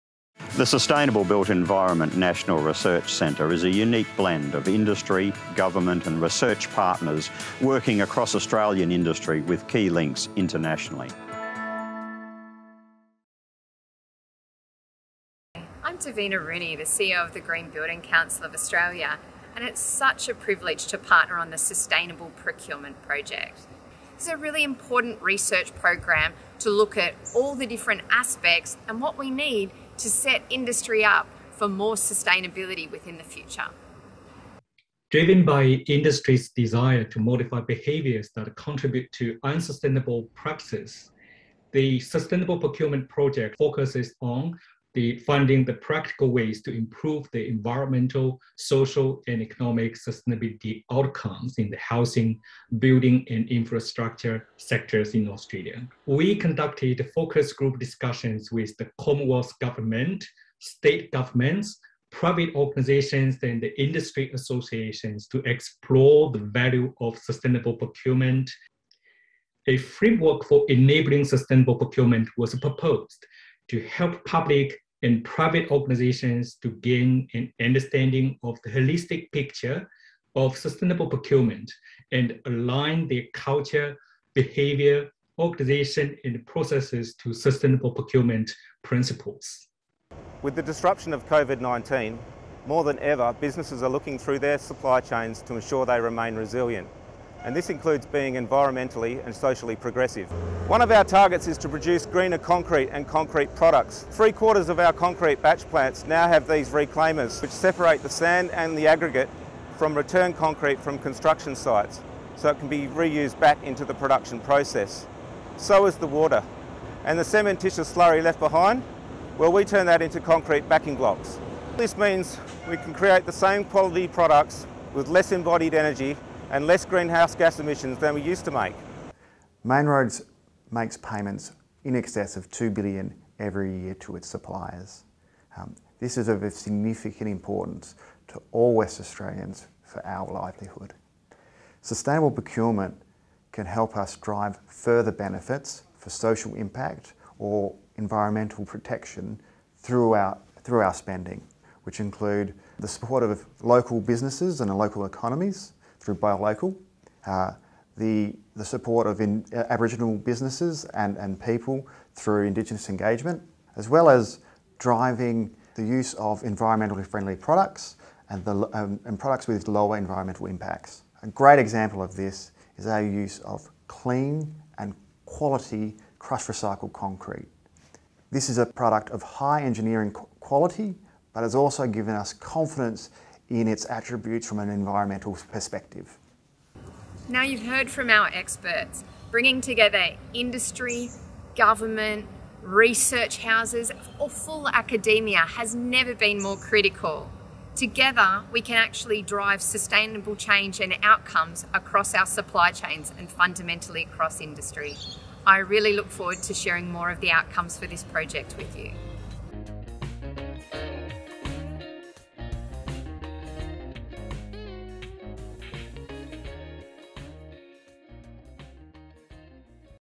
SBEnrc P2.76 Sustainable Procurement - Short Summary
This video is a short summary of the outcomes of research carried out by the Sustainable Built Environment National Research Centre (SBEnrc) as part of 2.76 Sustainable Procurement.